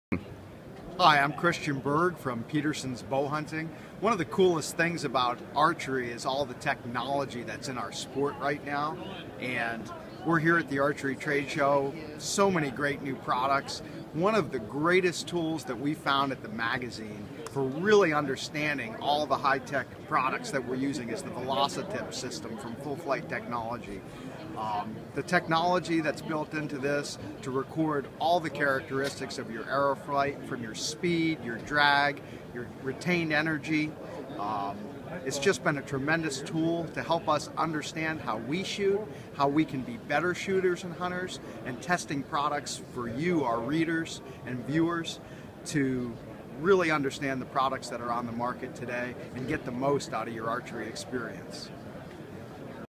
Petersen's Bowhunting Editor Christian Berg in Nashville at the 2014 ATA
Petersen's Bowhunting Editor Christian Berg discusses the technology of archery at the 2014 ATA Show in Nashville